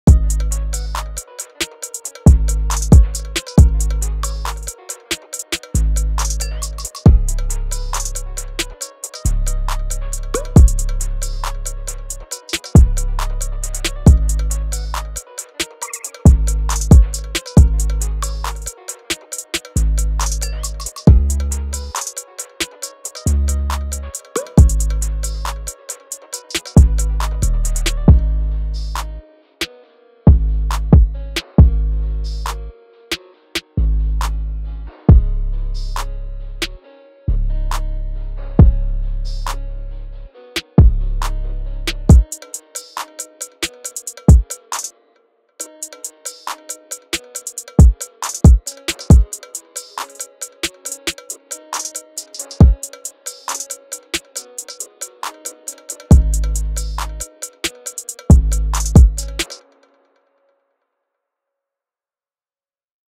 Retro Gaming Hip Hop Beat
Made A Hip Hop Beat With A Retro Gaming Sample
This Is A Preview Of The Beat
Comment In The Comment Section If YOu Want The Full Beat